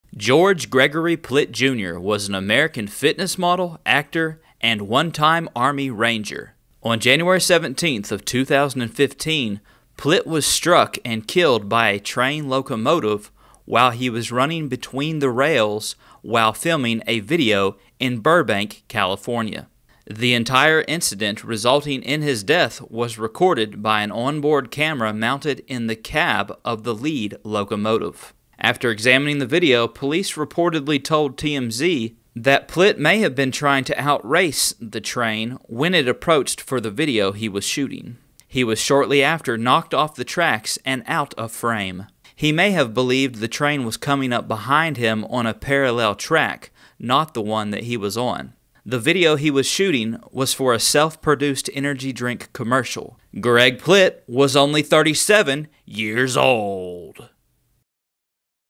How Did Greg Plitt Die?
George Gregory Plitt, Jr. was an American fitness model, actor, and onetime Army Ranger.